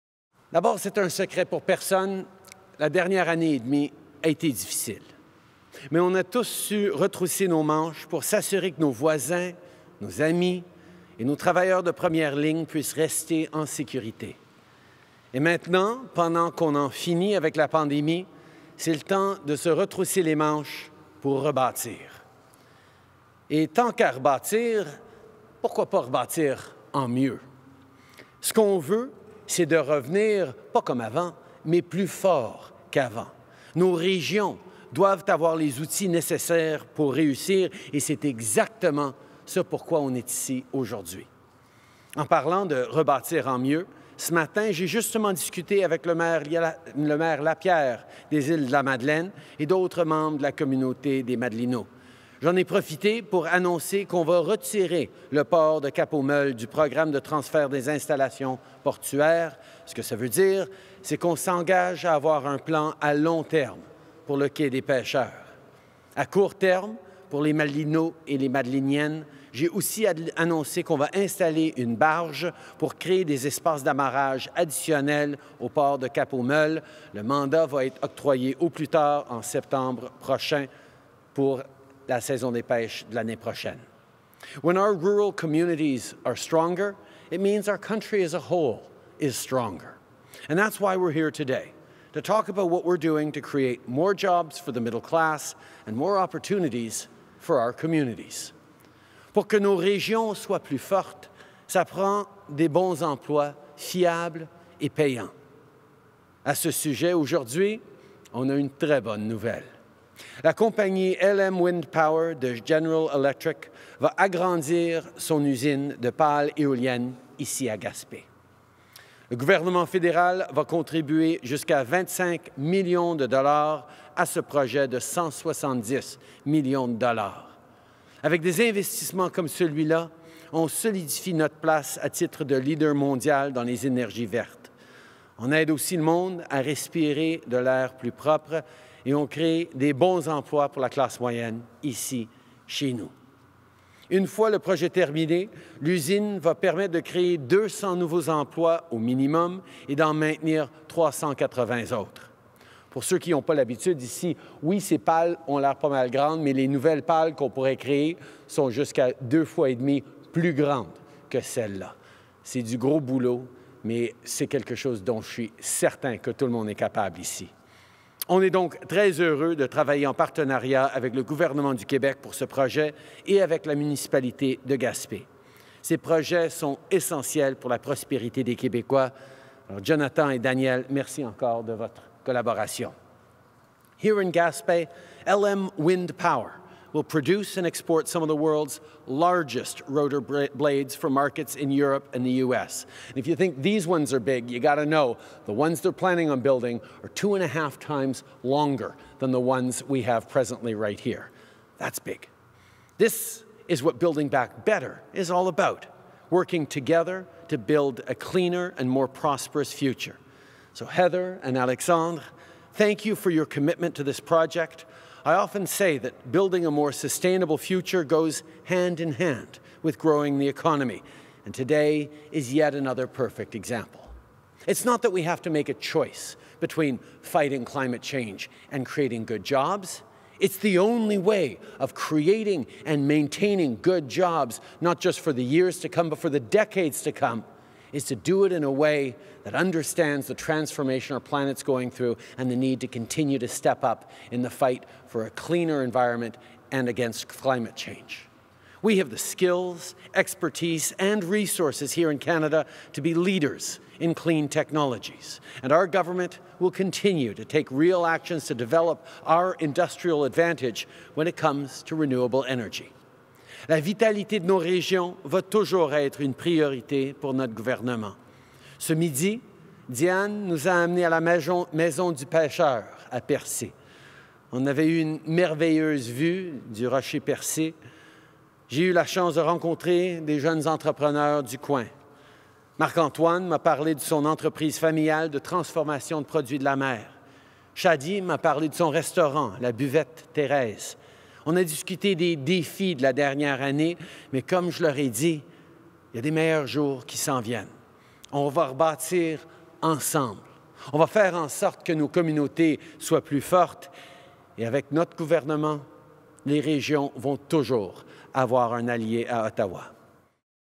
Support for renewable energy manufacturing in Quebec to create jobs and build a cleaner future
Prime Minister Justin Trudeau delivers remarks announcing support for renewable energy manufacturing in Quebec to create jobs and build a cleaner future.